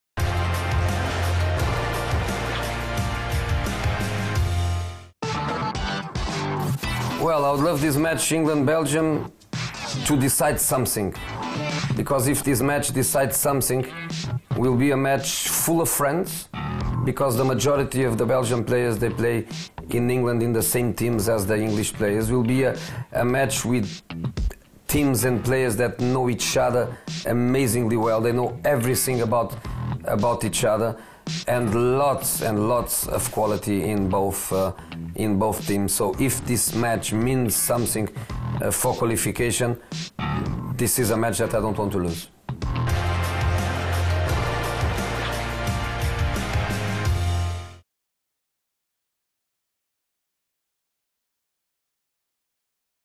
Jose Mourinho Predicts The Result Of The England Versus Belgium Match | Sportskeeda
Jose Mourinho talks about the game between England and Belgium!